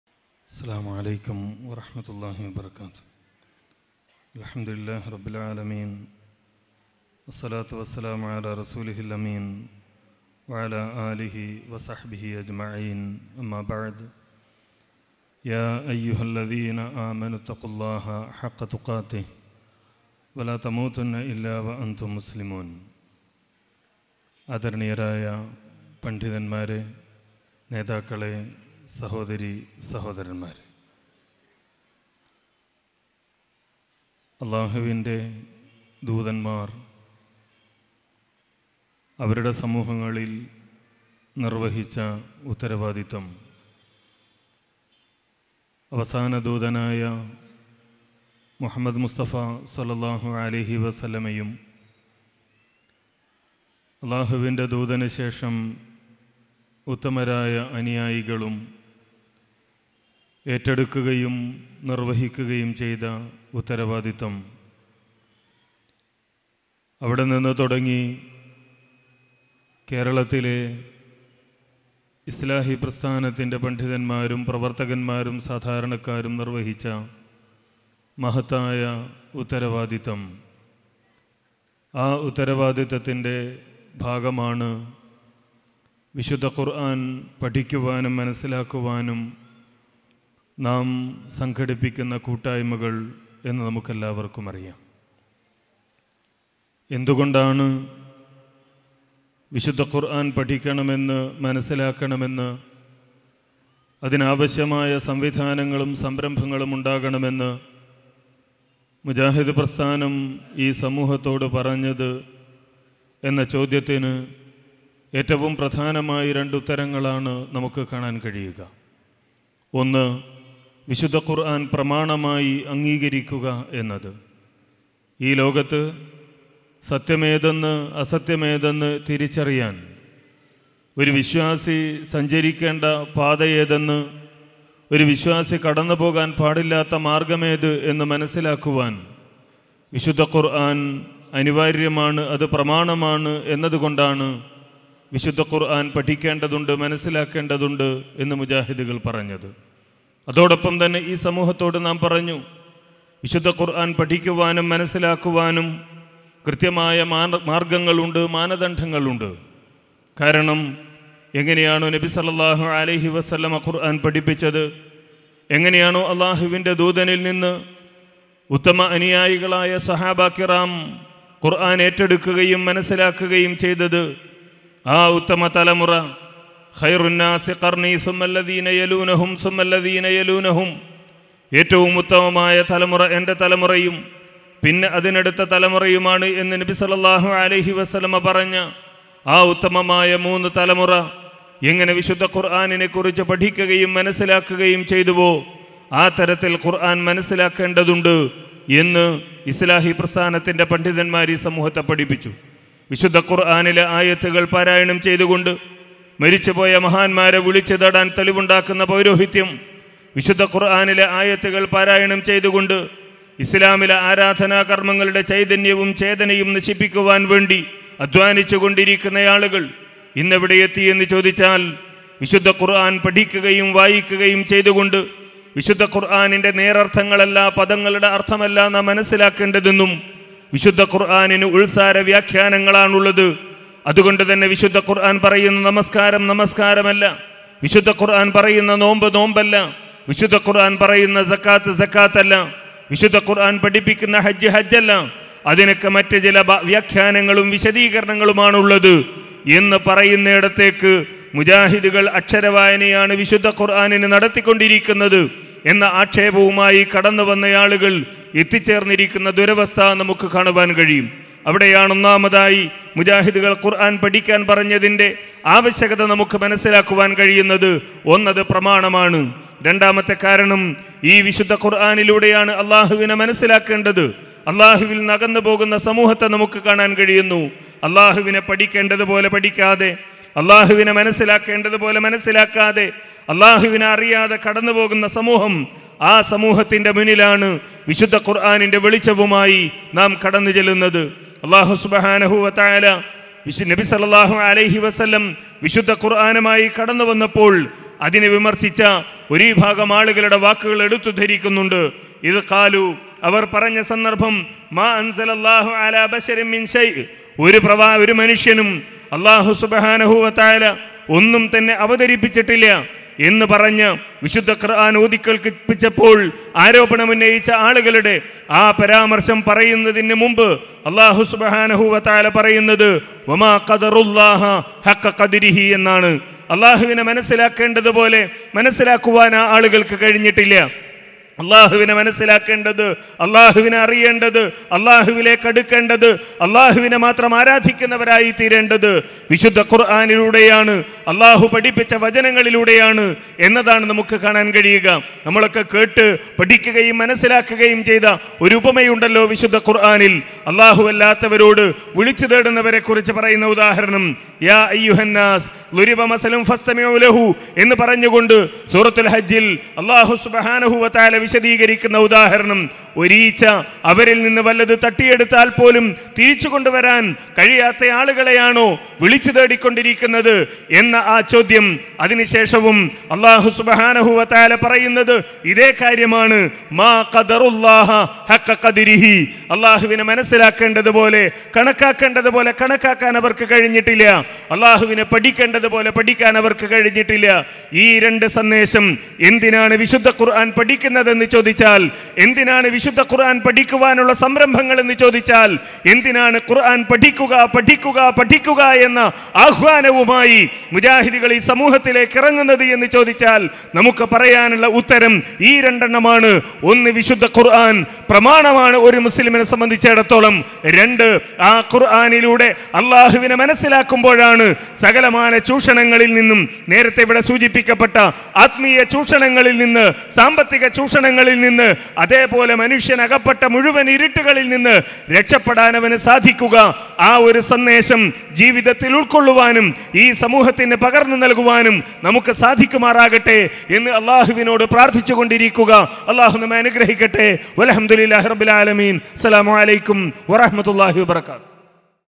ISM KERALA | വെളിച്ചം | സമാപന സമ്മേളനം | ഹനീഫ് കായക്കൊടി
Ism Kerala | Velicham | Sixth stage | Haneef Kayakkodi

ഖുർആൻ നേരിന്റെ നേർവഴി
വെളിച്ചം
ഖുർആൻ അന്താരാഷ്ട്ര പഠന പദ്ധതി ആറാം ഘട്ടം
സംസ്ഥാന സംഗമം
2018 സെപ്റ്റംബർ 8 ശനി
വടകര ടൌൺ ഹാൾ

ഹിദായ മൾട്ടീമീഡിയ പുറത്തിറക്കുന്ന പ്രോഗ്രാമുകൾ നിങ്ങളുടെ മെയിലിൽ ലഭിക്കുവാൻ താഴെയുള്ള ലിങ്കിൽ ക്ലിക്ക് ചെയ്യുക.